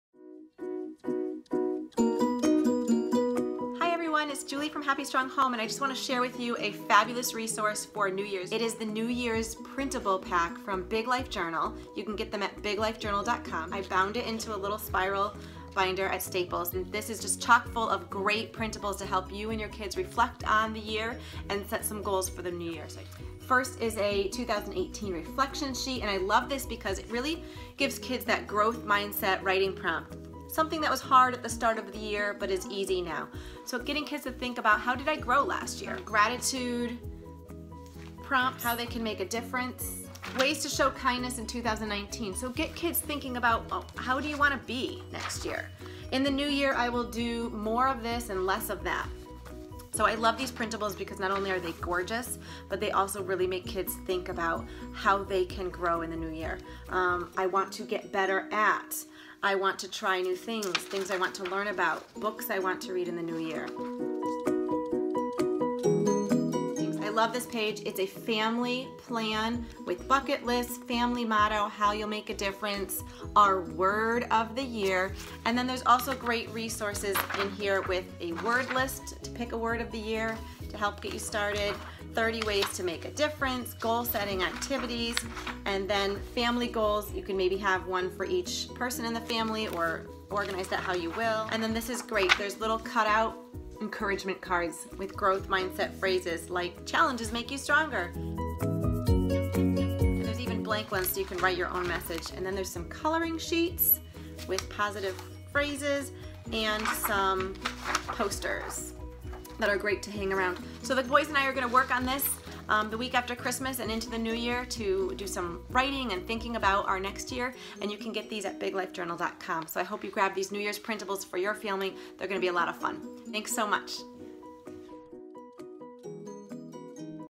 New Year's Printables from Big Life Journal
New Year's Printables from Big Life Journal give parents and kids a gorgeous way to reflect and set goals for the new year! Complete these as a family and refer to your goals throughout the year. Growth mindset prompts and charts to fill out on beautiful watercolor designed pages! (Review copy received from Big Life Journal)